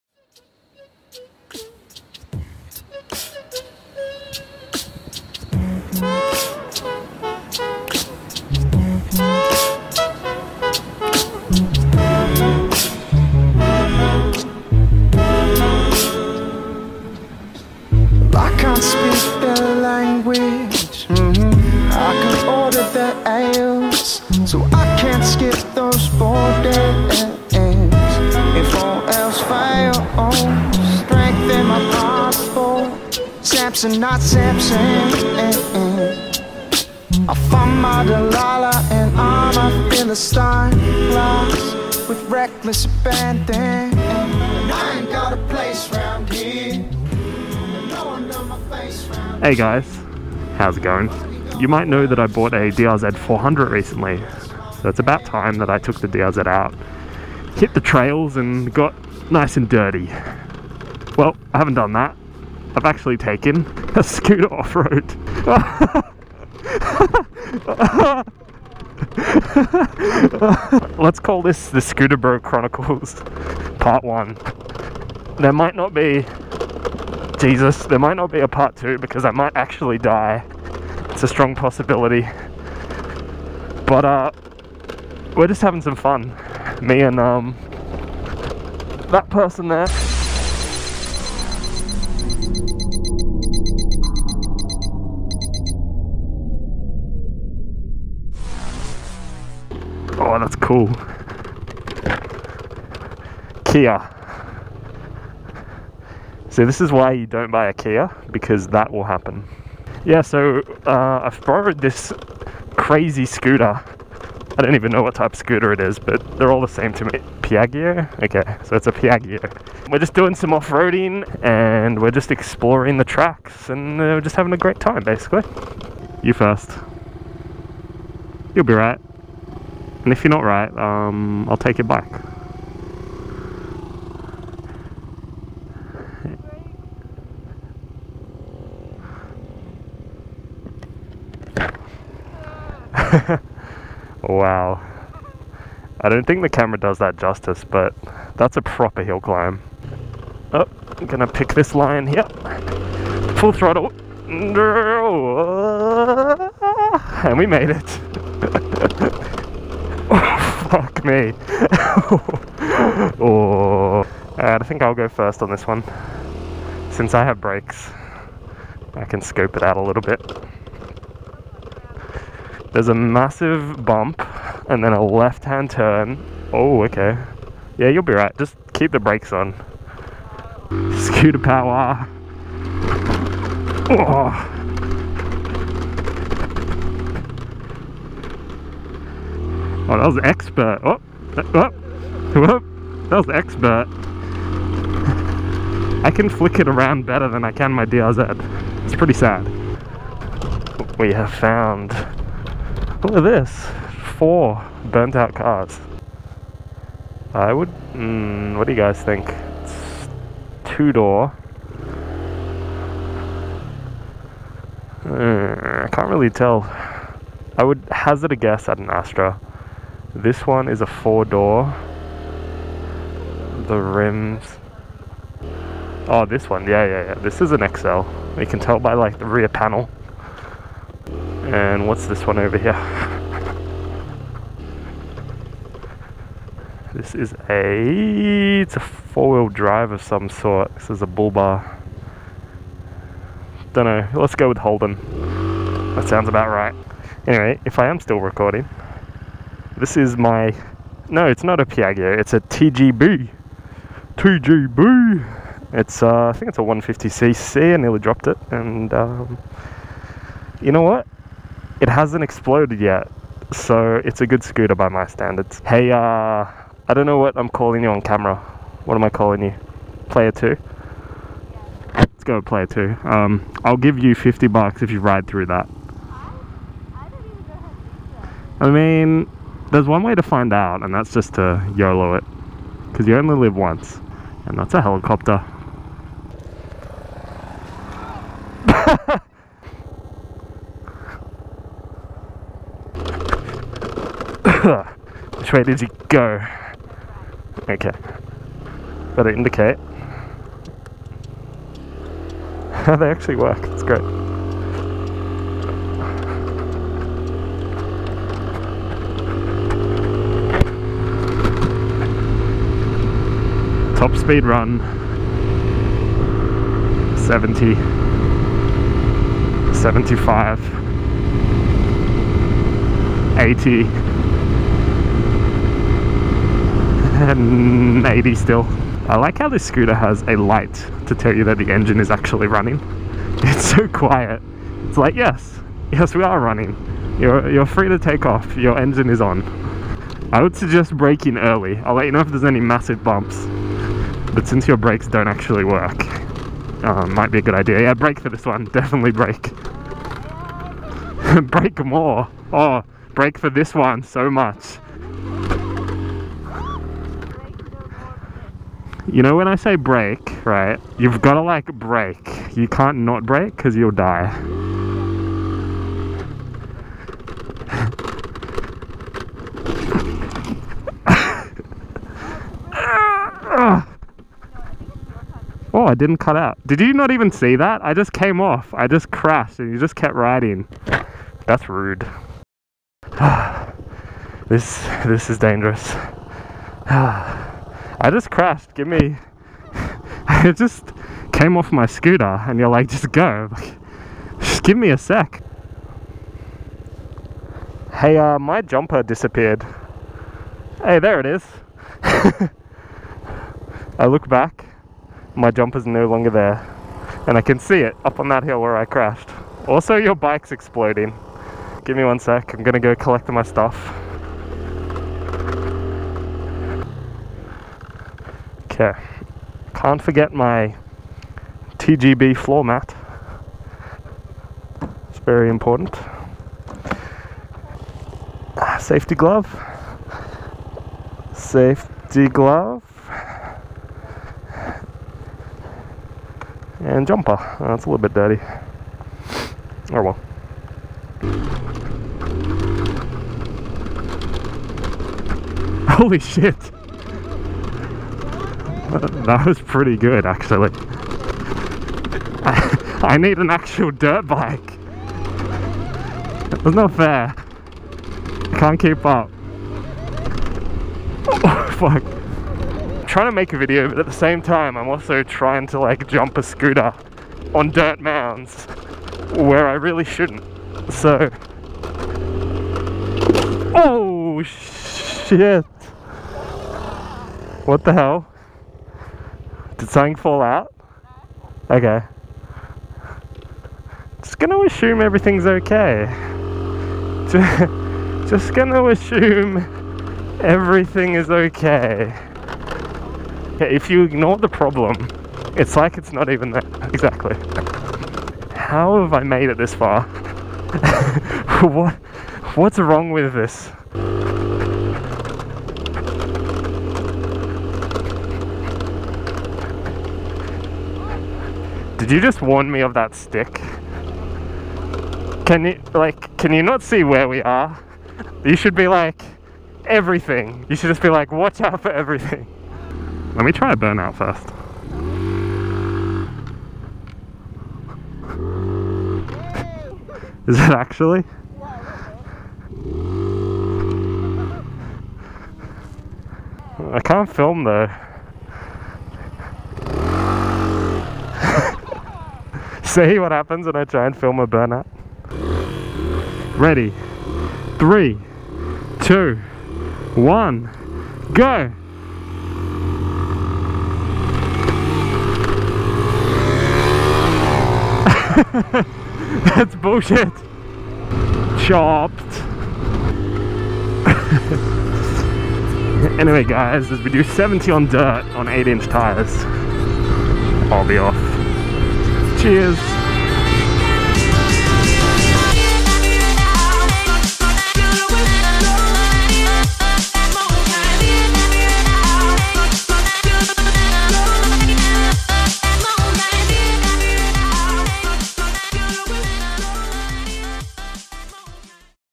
The ULTIMATE SCOOTERBRO EXPERIENCE
I crash a scooter while being the ultimate Scooterbro!

"What's the worst that could happen?"

▶ MUSIC

Vlog / motovlog / motovlogging in Sydney on a Honda CBR1000RR.

Basically funny motocross.